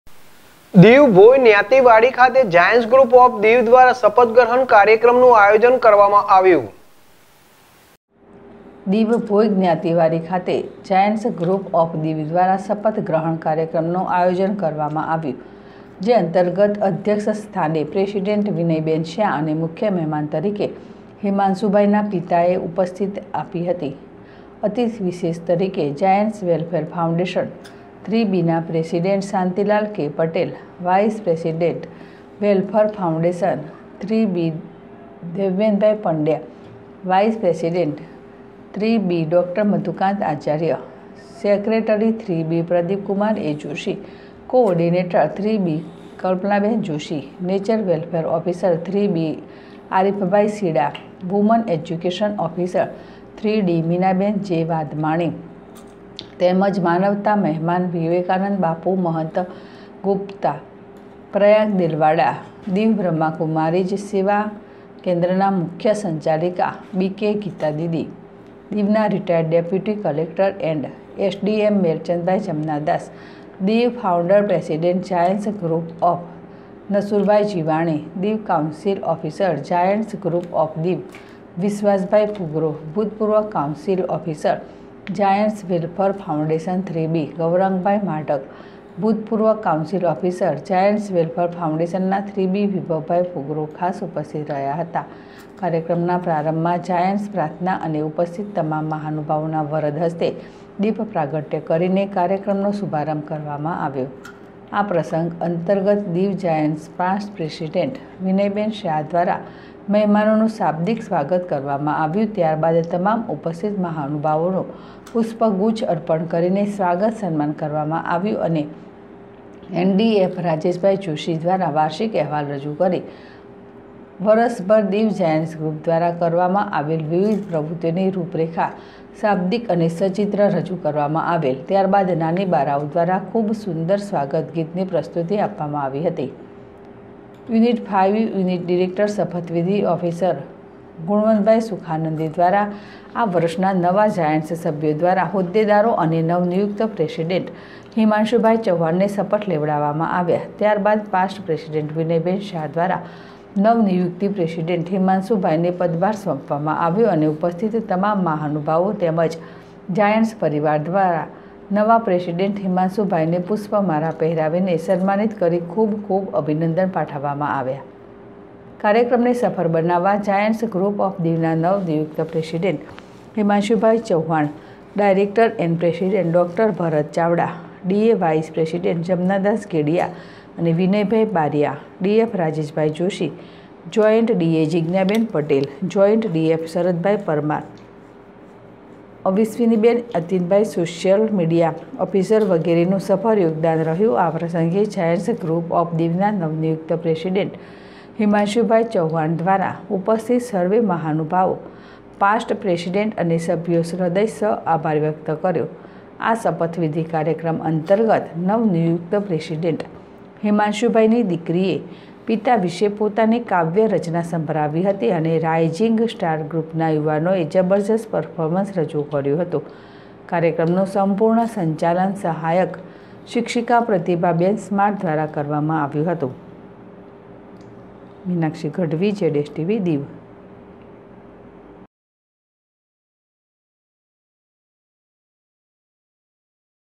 જાયન્સ ગ્રુપ દિવ દ્વારા ૧૬મો શપથવિધિ સમારોહ યોજાયો
#: દિવ જાયન્સ ગ્રુપ દિવ દ્વારા ૧૬મો શપથવિધિ સમારોહ યોજાયો